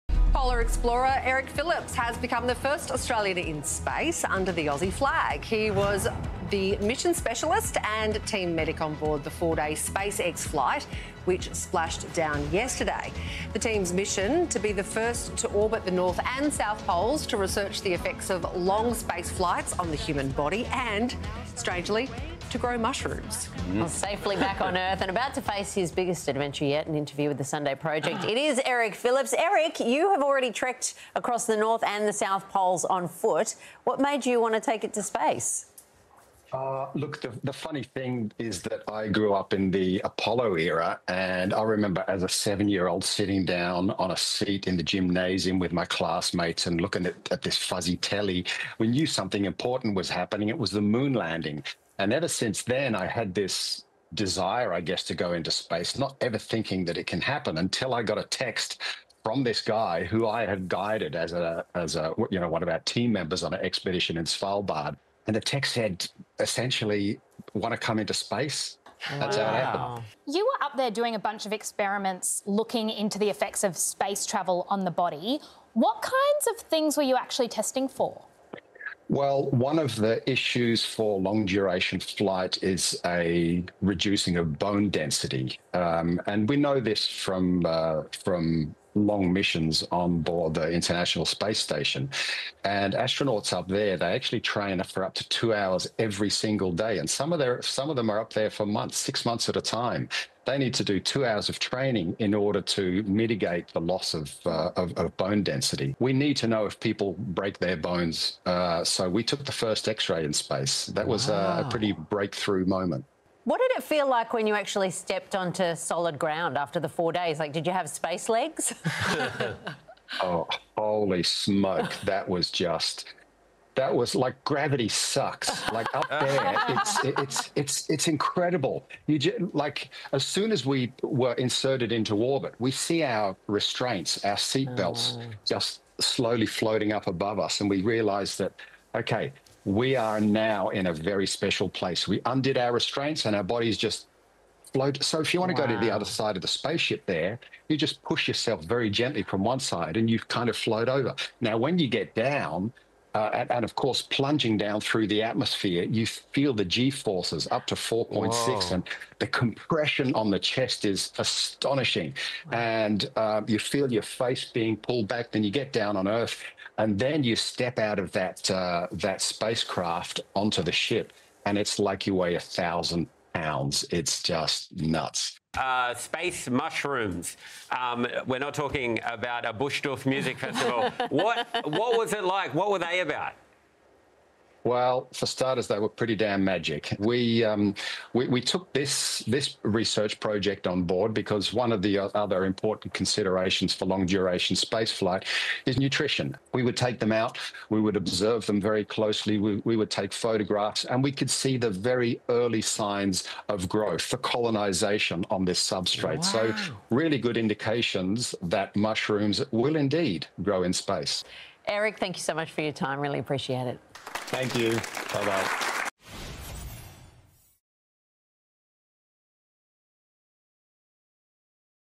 Aussie Polar Explorer Makes History In Space
Polar explorer Eric Philips became the first Australian in space under the Aussie flag, as part of research on the effects of long space flights on the human body... and to grow mushrooms.

He joined us to share everything about his four-day space flights.